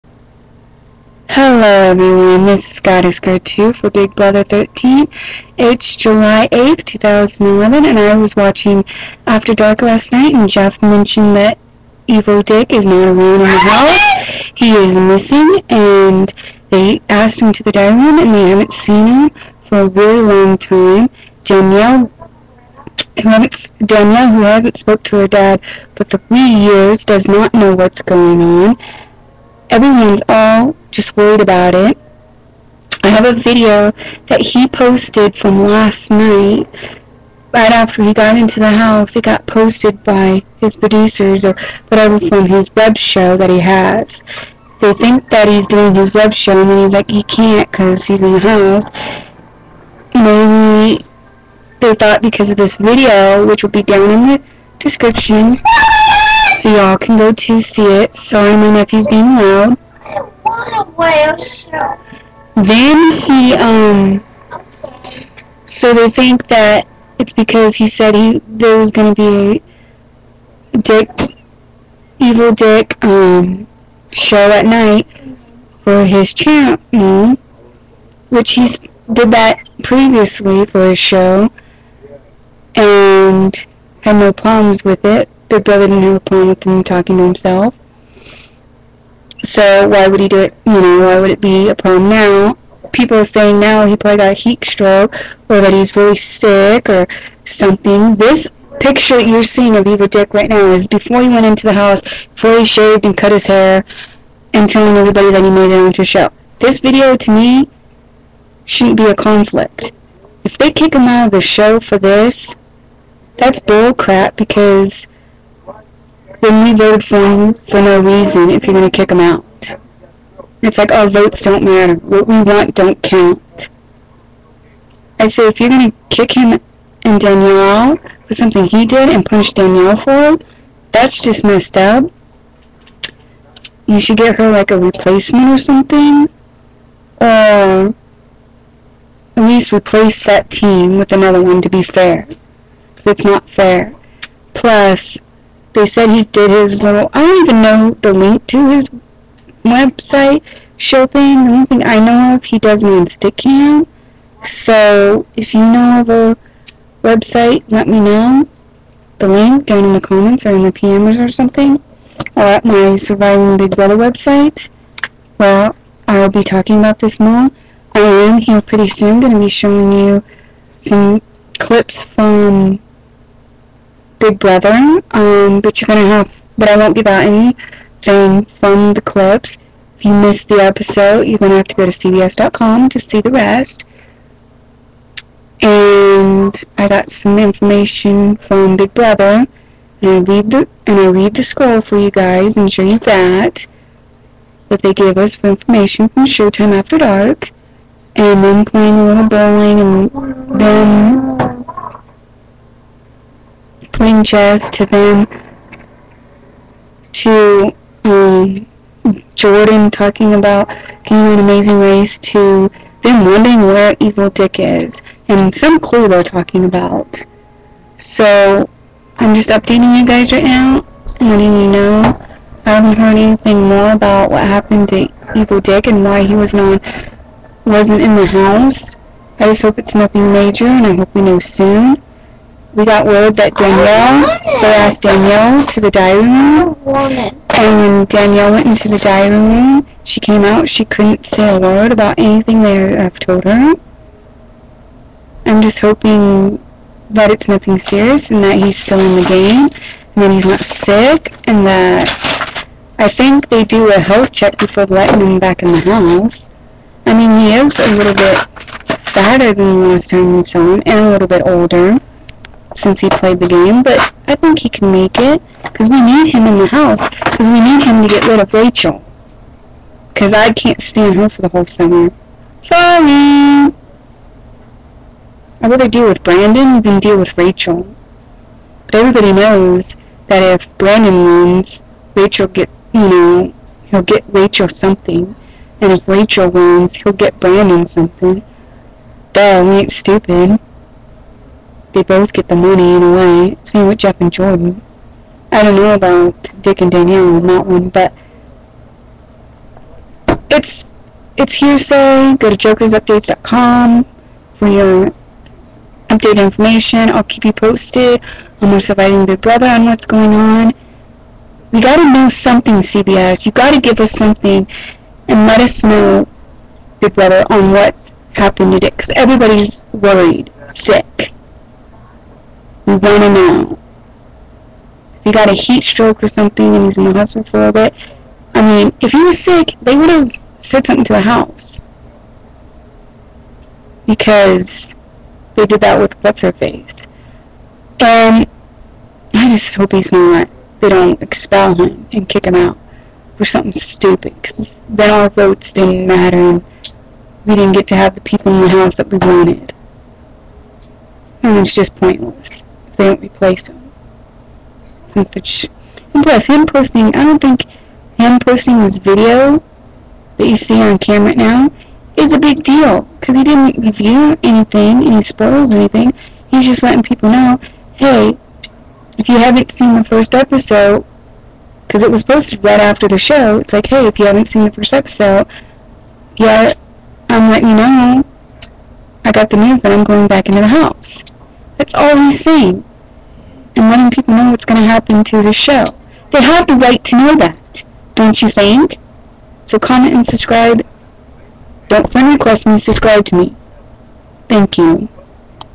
bb13 update on evel dick 7-8-11
HOH: Rachel NOM: Keith and Porsche
NEWS: Dick has left the game. Details are not clear yet on why, or how this affects the game.